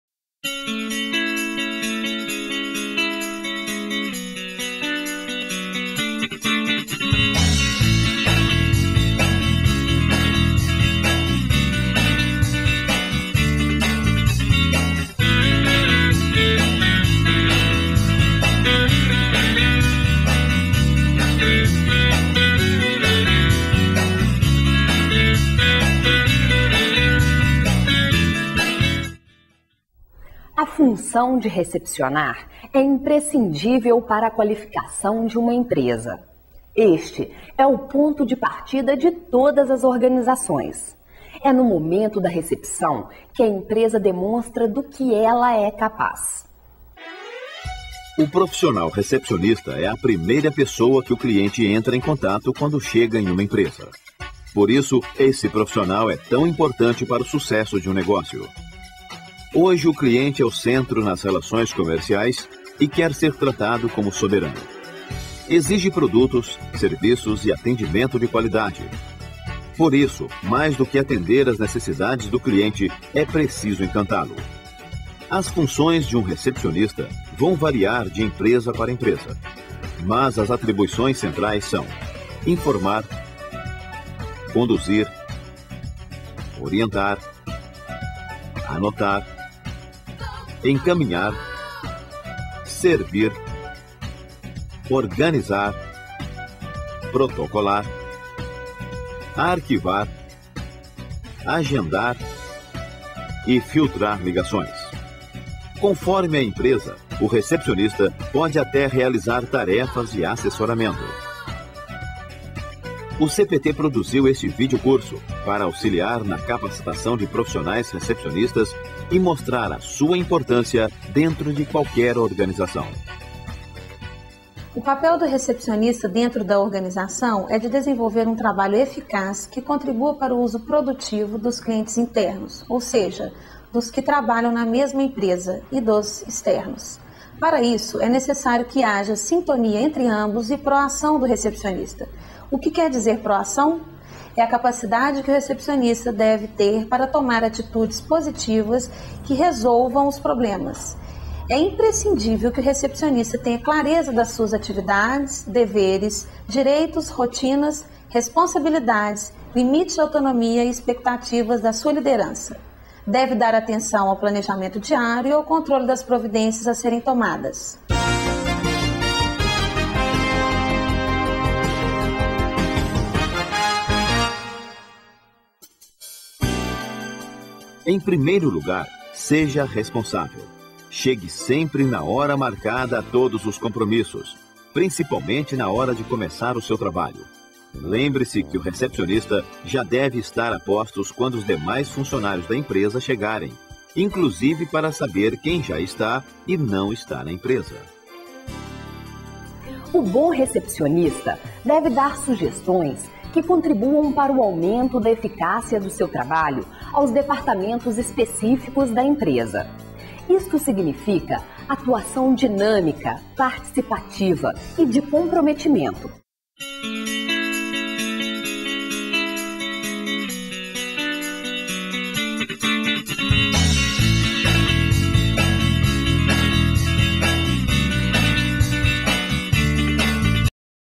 Curso Treinamento de Recepcionista - Cursos CPT
Este é um demonstrativo do filme que compõe o curso CPT Treinamento de Recepcionista. O Curso completo é constituído de manual com 90 páginas e, em DVD com total de 67 minutos.
O curso aborda: função do recepcionista, perfil do recepcionista, planejamento das atividades, controle das atividades, marketing pessoal, comunicação produtiva, relacionamento interpessoal, atendimento eficaz e sugestões para o sucesso.